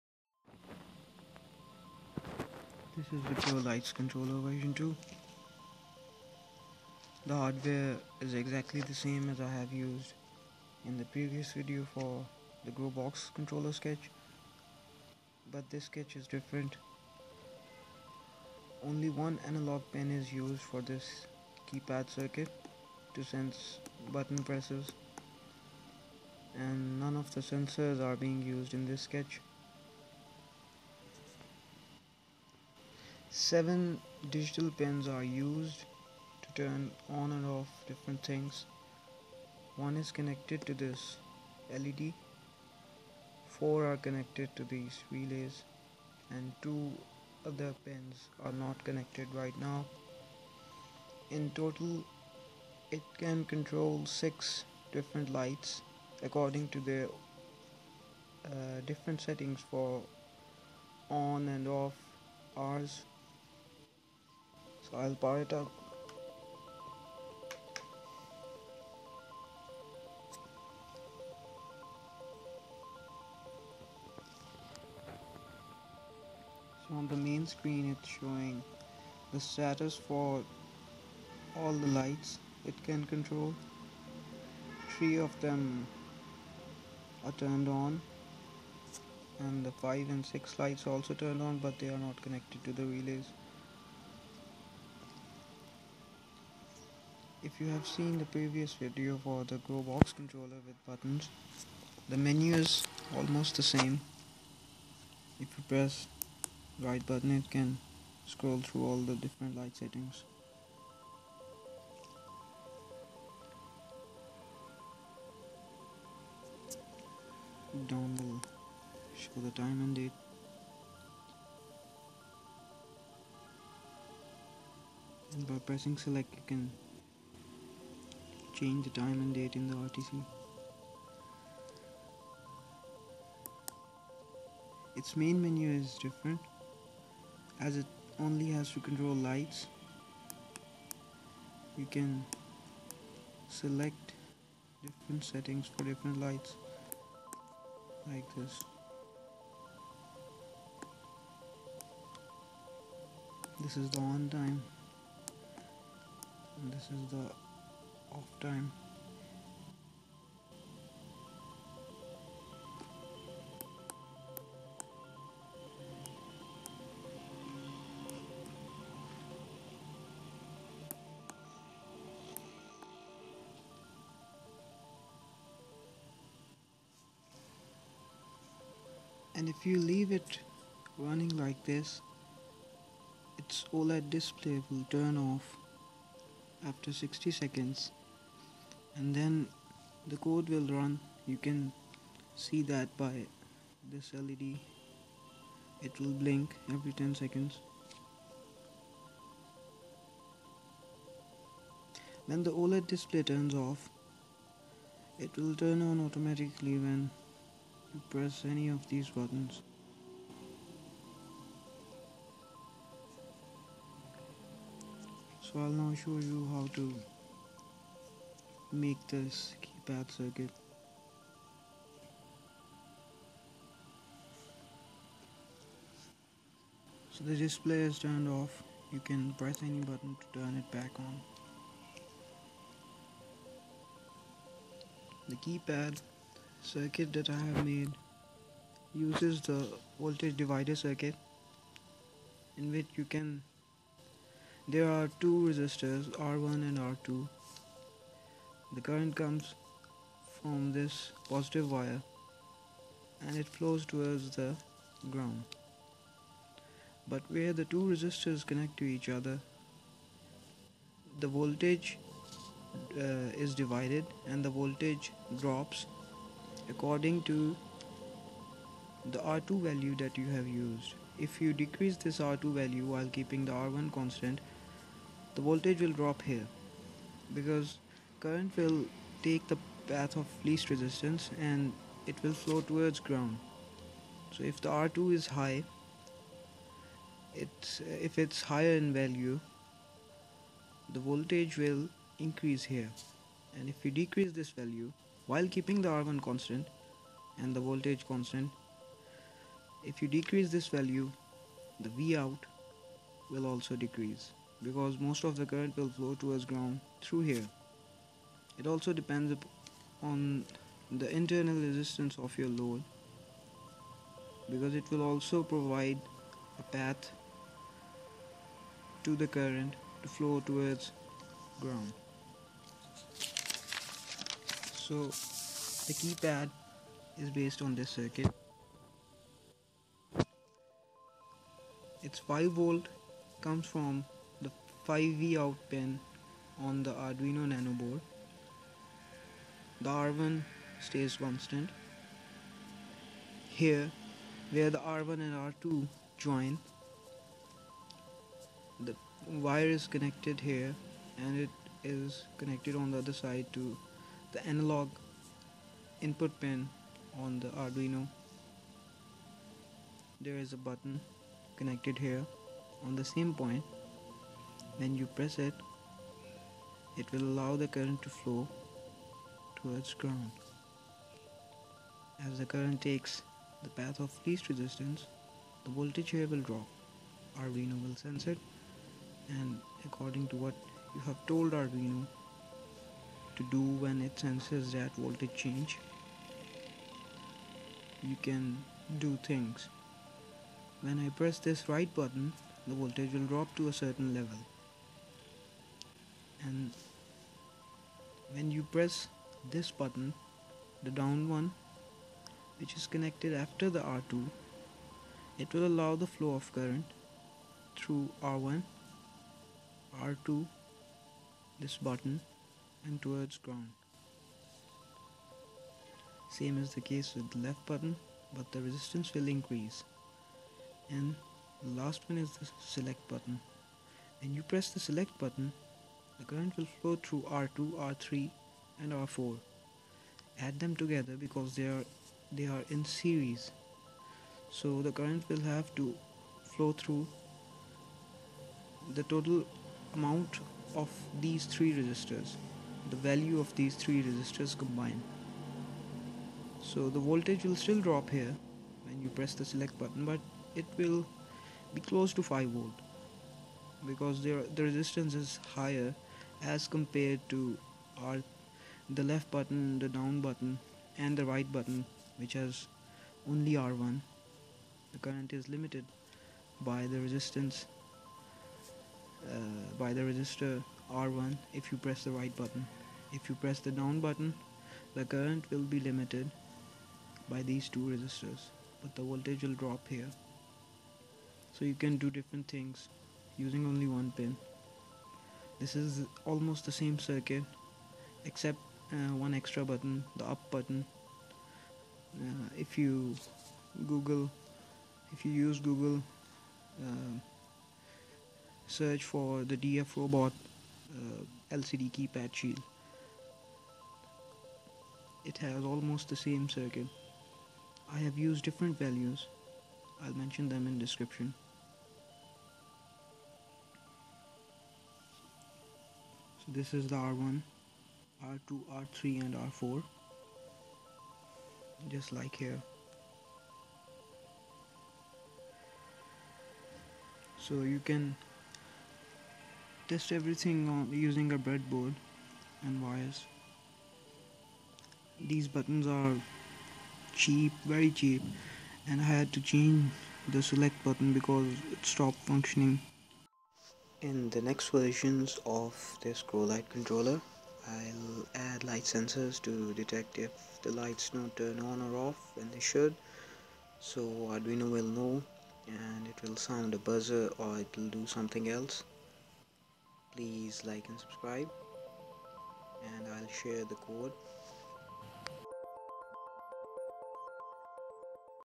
Grow Lights Controller with Oled how to make Keypad
Programmable Grow Lights Controller Timer for Indoor Environment with Oled DIY how to make Keypad for Arduino

New version has Two on/off timer settings for Each of Six Lights

Resistor Values For KeyPad :
   R1 = 2.1 Kilo Ohm
   R2 = 1 Kilo Ohm
   R3 = 1 Kilo Ohm
   R4 = 1 Kilo Ohm

IF AREF pin is NOT Connected to 3.3V. then

 Keypad can be Connected to 5V. or  3.3V and

In the sketch there is a line analogReference DEFAULT meaning arduino can measure 0-5 Volts .Read as value between 0-1024

BUT if AREF pin is Connected to 3.3V pin  then

in the Sketch analogReference must be set to EXTERNAL instead of DEFAULT

arduino will measure from 0-3.3V and anything above 3.3 will read 1024 using analogRead. so Keypad must be powered with 3.3V .

But for stability it's best to use 3.3 V to power keypad so even if the input voltage changes between 4.5-5 V the button voltages will not be effected.

# For Sketch/Code (with oled/buttons and without oled/buttons)

3. Send me your Gmail Address

Some Info about Switching Lights

For Normal Light bulbs you can use these kind of relays(blue ones in the video) and their current handling capacity is mentioned on the Relays (5AmpsAt220V).

** As a general rule never go near it's rated capacity and try to stay near  the half of its rated capacity . **

To Control High Powered  Lights you'll have to use powerful relays they can be mechanical or solid state but in both cases you'll have to Power them up to Switch them On or Off .

 FIRE and ELECTRIC SHOCK HAZARD must be kept in mind and
 should be a priority

Please open the folder 'All of my code' then the Grow Environment Controllers  '

Some relays need DC current and some need AC in both cases you can use this kind of Controller (along with  relays ) to Power up High Capacity Relays and then those High Powered ones will control your lights and will Turn them on /off .

Everything can be installed in a Metal Electric Panel Box with Relay sockets and Circuit Breakers all wired up neatly with proper Earth connection for safety,

So if you don't know what you are doing then consult an Electrician